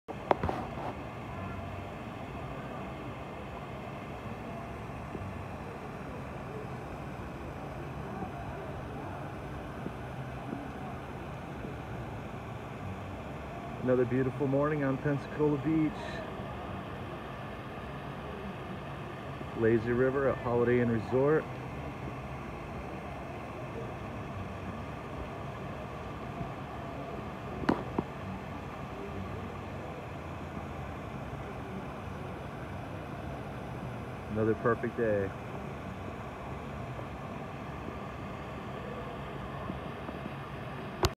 Pensacola Beach Report 08182021 | PensacolaBeach.com
It sounds a little bit like a broken record, but WOW! What another gorgeous day on Pensacola Beach.  The water is super calm and the weather is perfect for a great day at the beach.  Lazy Days has all their chairs and umbrellas in a nice line right along the beach.  Come on down and enjoy!